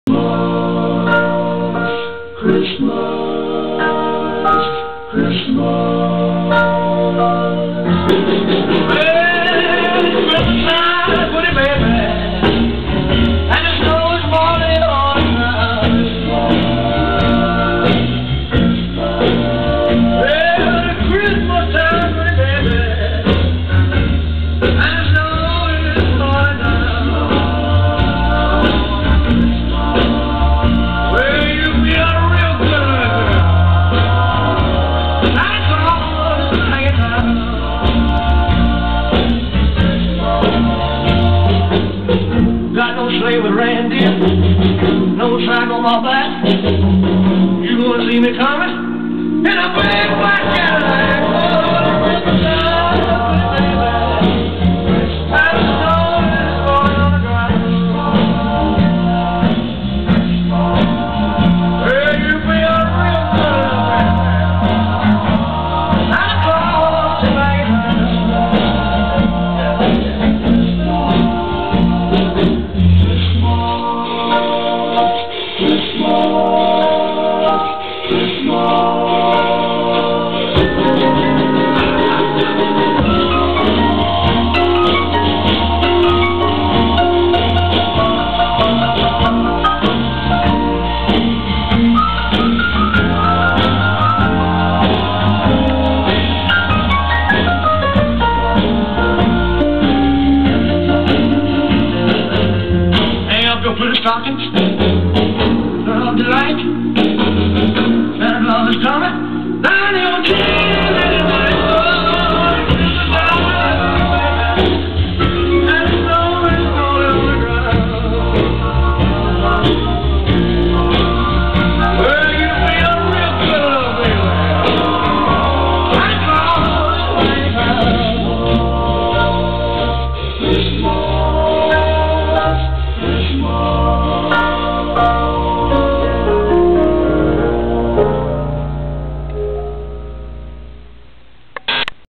Happy COLTSMAS Baby
My Colts filled living room including my COLTSMAS tree, signed pix , posters and football set to Christmas music